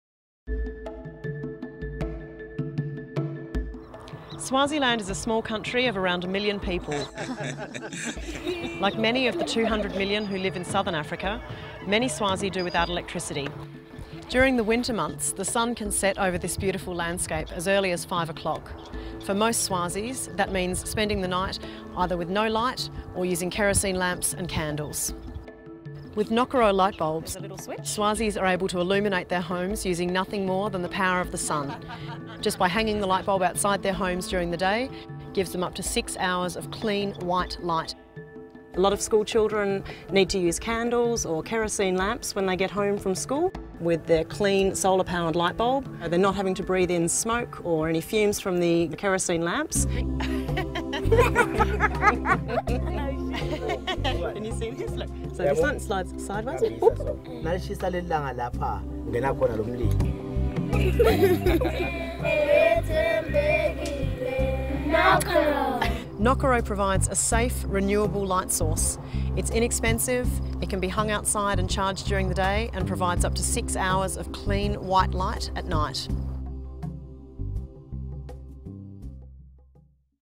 Nokero in Swaziland, Africa
Emmy-nominated filmmaker John Mans, together with filmmakers Andi Lucas and Melinda MacInnis, bring this inspiring video short showing how solar and renewable energy can help bring much-need light to Africa. Featuring the wild landscapes of Hlane Royal National Park and Mlilwane Game Reserve, the video shows the Nokero Solar Light Bulb in use by villagers there.